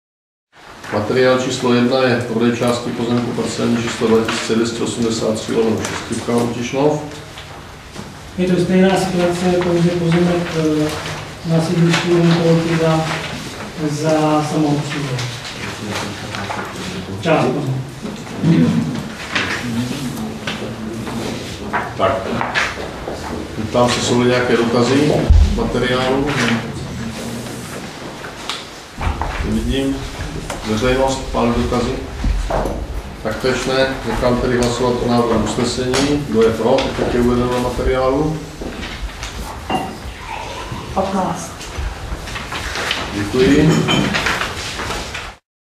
Zastupitelstvo města Tišnova 7.12.2015 - bod 31
Prodej části pozemku parc.č. 2283/6 v k.ú. Tišnov